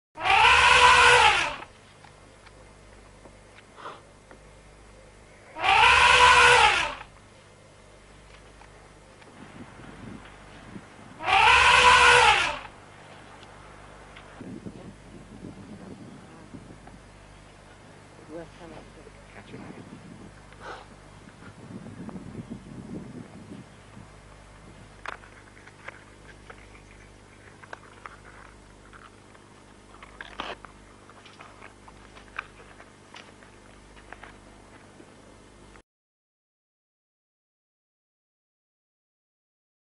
Huge Indian Elephant - Rajagaj
The clip was shot in Bardia, Nepal by an expedition led by Colonel John Blashford-Snell CBE in 1992 when they found an enormous elephant, 11 feet 3 inches at the shoulder and believed to be the largest in Asia.

Local people thought he was a mammoth, but he was in fact an Asian elephant. The Nepalese put him on a postage stamp and sent soldiers to guard the area. They are still there and this is one of the reasons that the tiger population has increased. The book “Mammoth Hunt” that I wrote with Rula Lenska tells the story.  A TV film was made and widely shown all over the world.

Sadly, Raja Gaj disappeared in 2010 at the age of about 70. We believe he died and was swept away in the flooded Karnali river. However, his offspring are thriving and look like him.